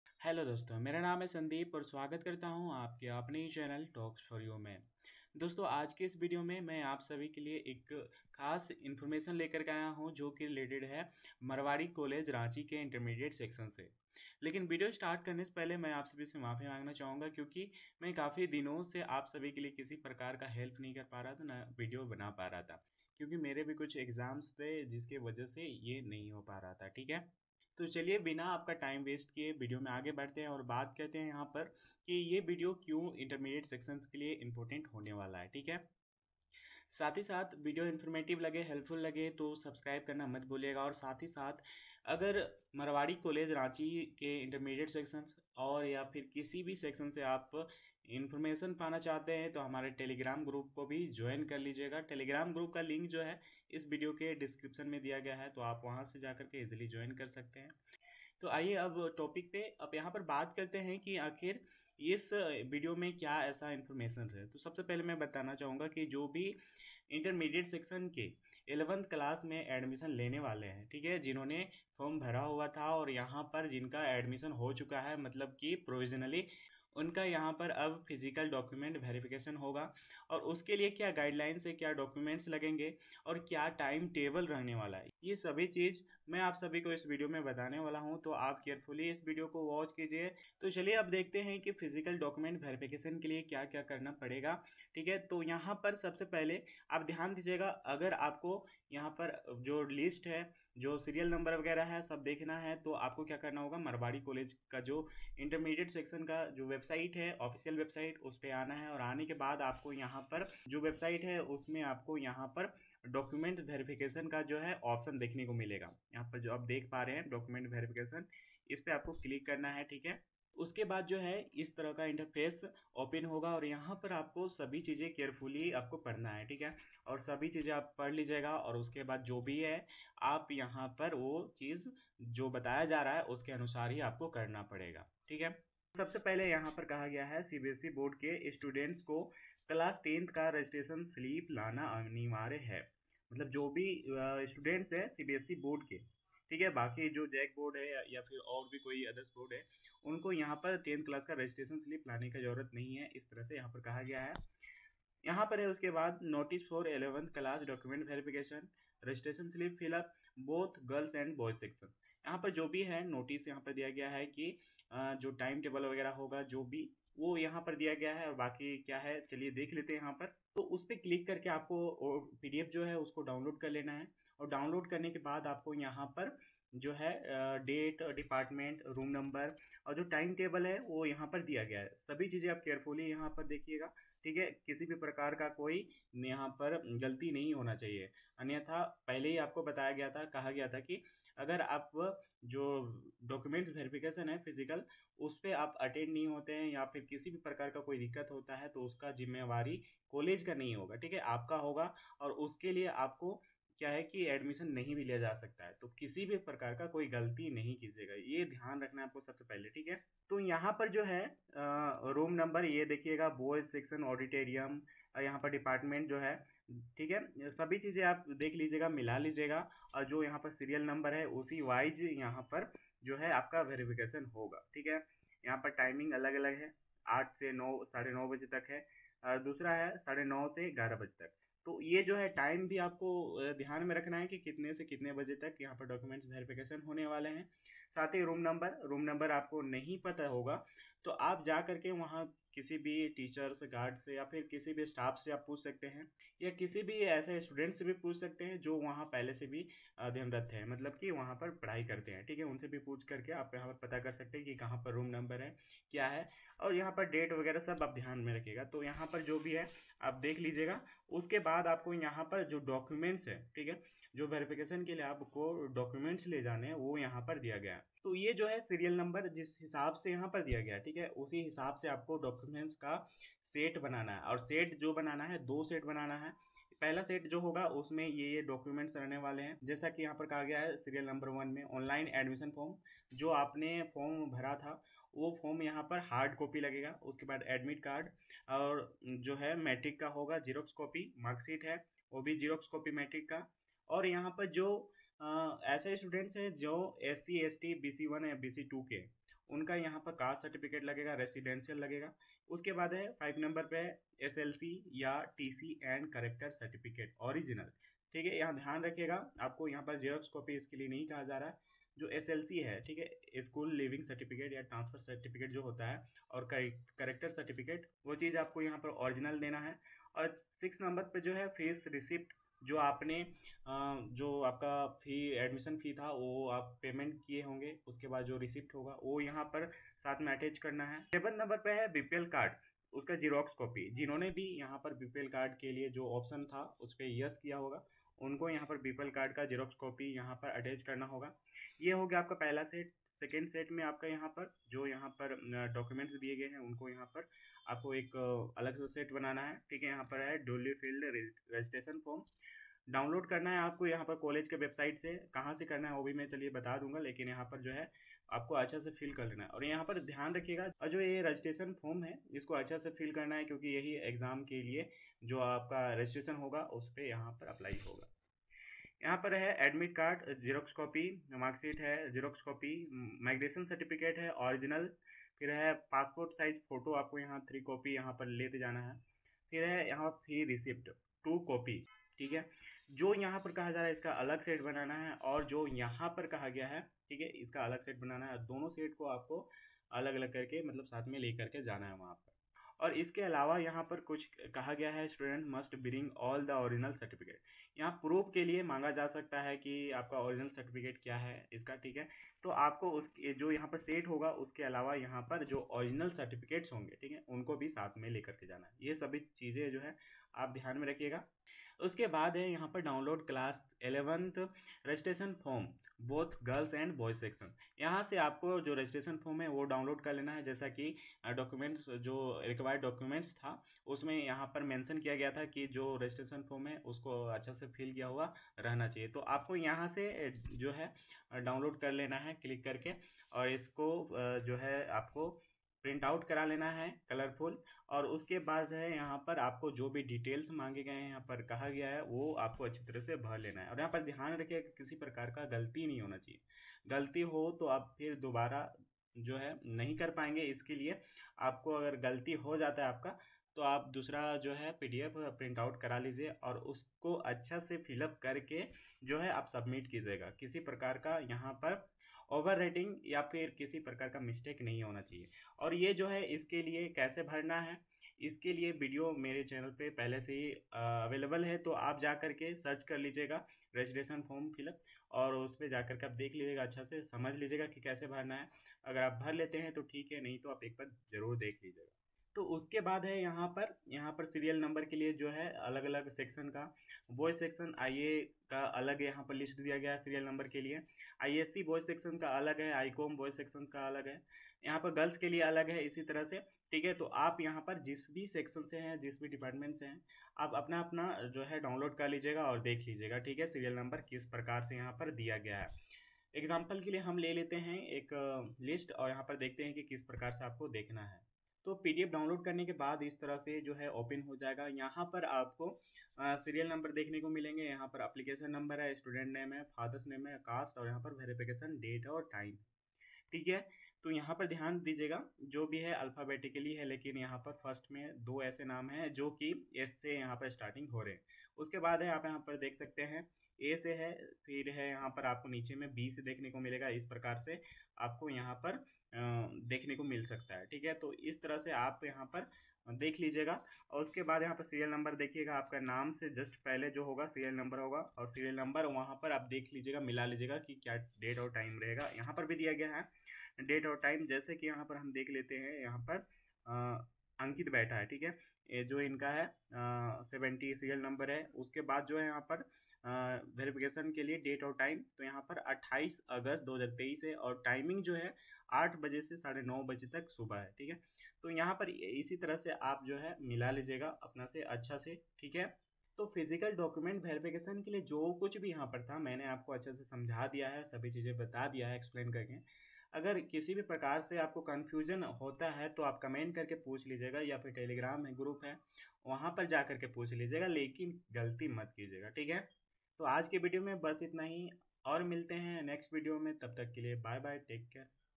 Marwari College Ranchi 11th Class Physical Document Verification | Boys and Girls, I.Sc, I.A, I.Com
Marwari College Ranchi Intermediate Physical Document Verification, Boys and Girls, I.Sc, I.A, I.Com

Marwari College Ranchi

Intermediate Physical Document Verification

Serial number I.Sc. Boys

Serial number I.Com. Boys

Regards
NKP Production
Neeraj Kumar
Joint secretary MCRSU (19-20)